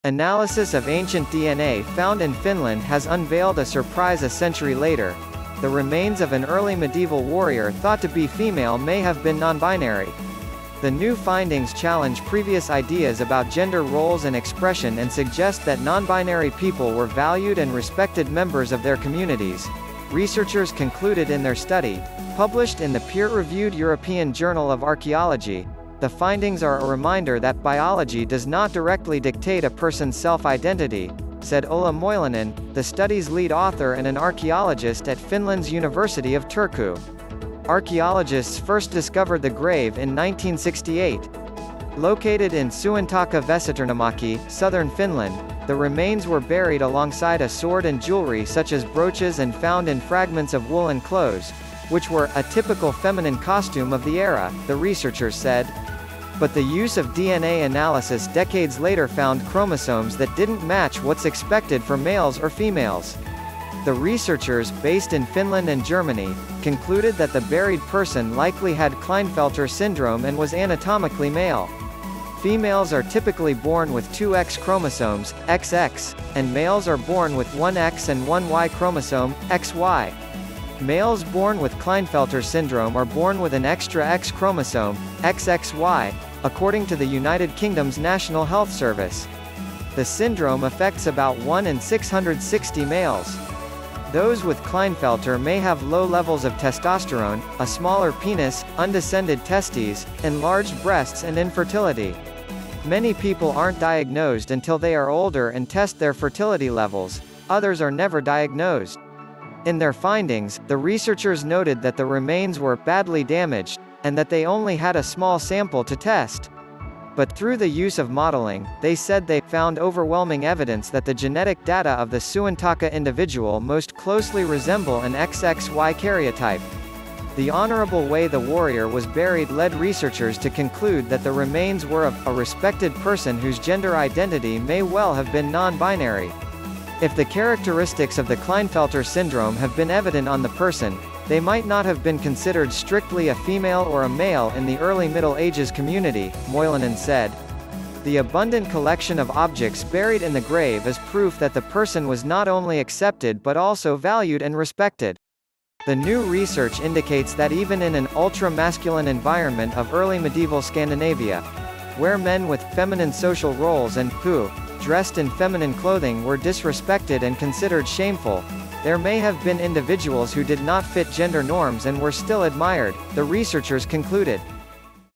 1000 Year Old Remains May Be Of A Highly Respected Nonbinary Warrior
Analysis of ancient DNA found in Finland has unveiled a surprise a century later – the remains of an early medieval warrior thought to be female may have been nonbinary. The new findings challenge previous ideas about gender roles and expression and suggest that nonbinary people were valued and respected members of their communities, researchers concluded in their study, published in the peer-reviewed European Journal of Archaeology,The findings are a reminder that "biology does not directly dictate a person's self-identity," said Ulla Moilanen, the study's lead author and an archaeologist at Finland's University of Turku. Archaeologists first discovered the grave in 1968. Located in Suontaka Vesitorninmäki, southern Finland, the remains were buried alongside a sword and jewelry such as brooches and found in fragments of woolen clothes — which were "a typical feminine costume of the era," the researchers said. But the use of DNA analysis decades later found chromosomes that didn't match what's expected for males or females. The researchers — based in Finland and Germany — concluded that the buried person likely had Klinefelter syndrome and was anatomically male. Females are typically born with two X chromosomes (XX) and males are born with one X and one Y chromosome (XY). Males born with Klinefelter syndrome are born with an extra X chromosome (XXY), according to the United Kingdom's National Health Service. The syndrome affects about 1 in 660 males. Those with Klinefelter may have low levels of testosterone, a smaller penis, undescended testes, enlarged breasts and infertility. Many people aren't diagnosed until they are older and test their fertility levels; others are never diagnosed. In their findings, the researchers noted that the remains were "badly damaged" and that they only had a small sample to test. But through the use of modeling, they said they "found overwhelming evidence that the genetic data of the Suontaka individual most closely resemble an XXY karyotype."The honorable way the warrior was buried led researchers to conclude that the remains were of "a respected person whose gender identity may well have been non-binary.""If the characteristics of the Klinefelter syndrome have been evident on the person, they might not have been considered strictly a female or a male in the Early Middle Ages community," Moilanen said. "The abundant collection of objects buried in the grave is proof that the person was not only accepted but also valued and respected."The new research indicates that even in an "ultra-masculine environment of early medieval Scandinavia" where men with "feminine social roles and [who] dressed in feminine clothing were disrespected and considered shameful," there may have been individuals who did not fit gender norms and were still admired, the researchers concluded.